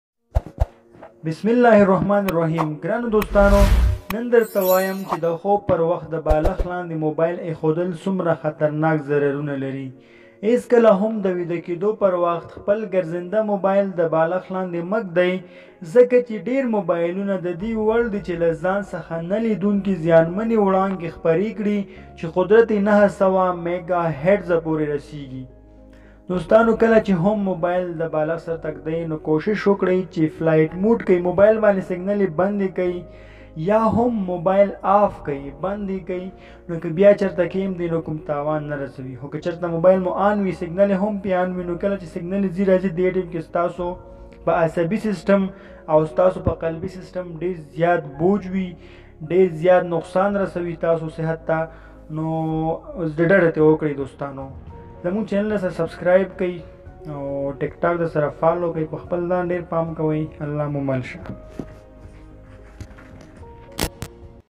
د خوب پر وخت موبایل د بالښت لاندی ایښودل ایا تاسی هم دا غلطی کوئ؟
موبایل
معلومات